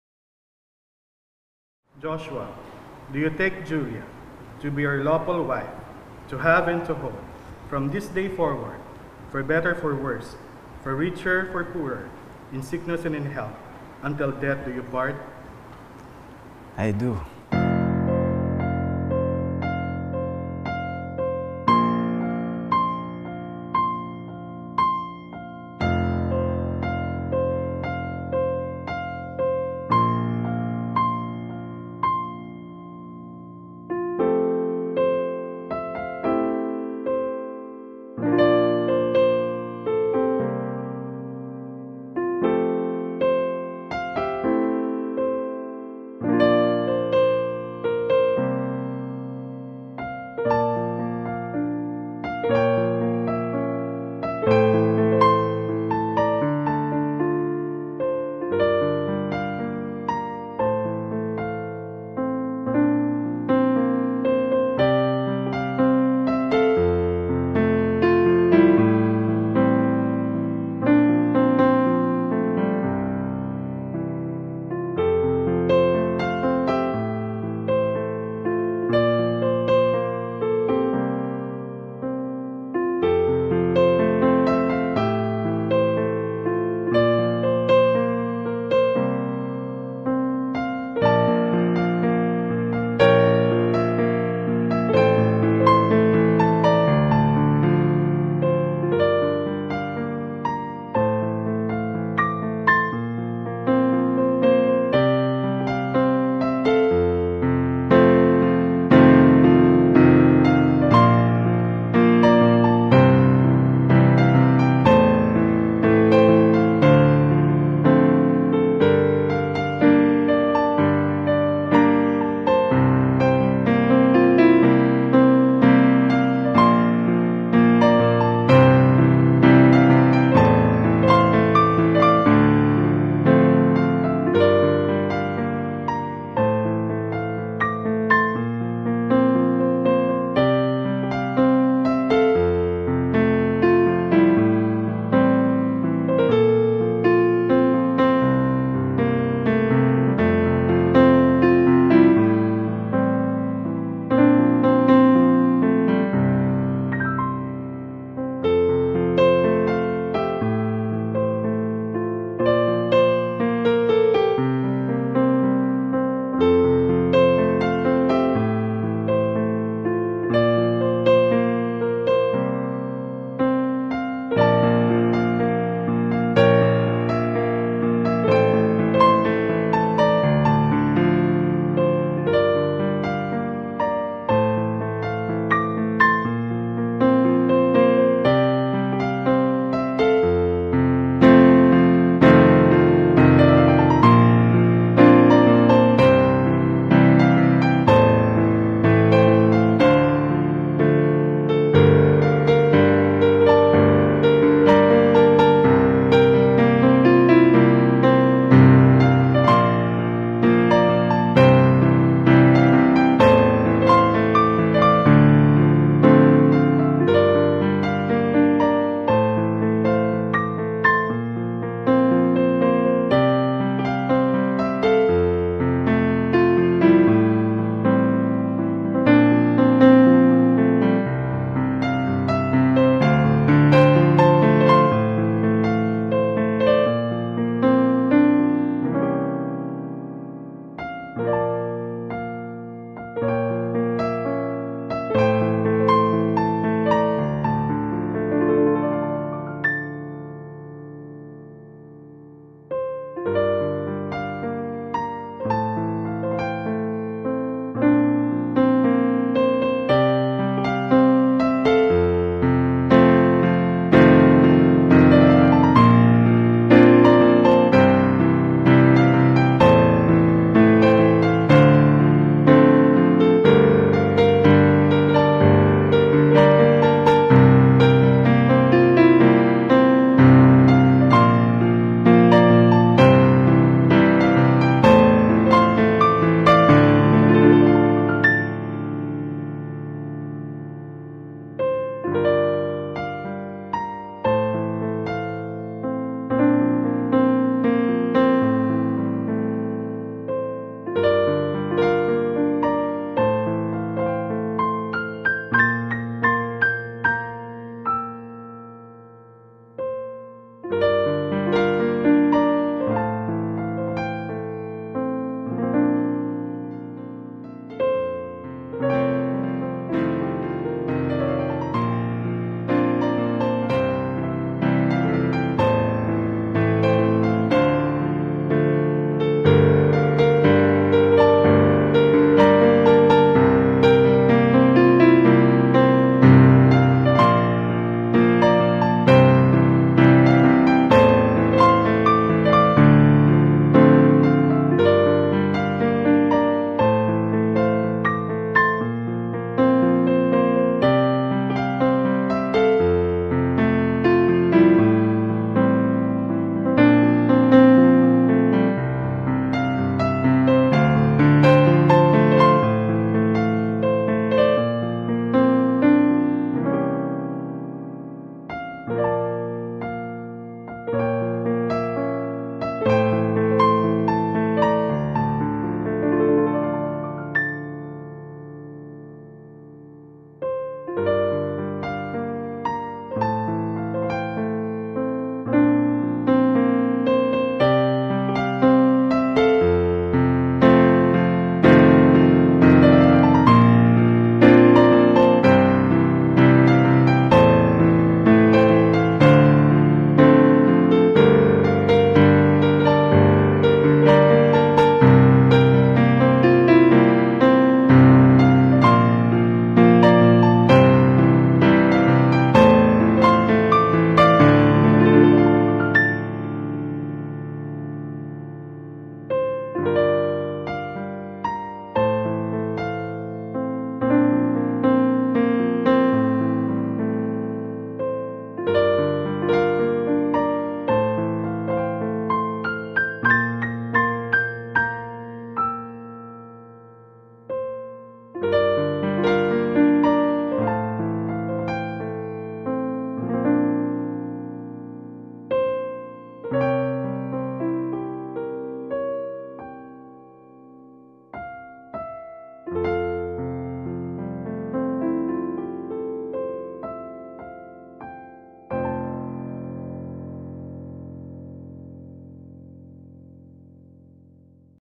JOSHLIA - DRAWING | TIME-LAPSE | JAYSON MOGELLO
JOSHLIA - DRAWING | JAYSON MOGELLO
I was inspired to draw this not because I am broken, but because I just felt their broken hearts when they ended their relationship.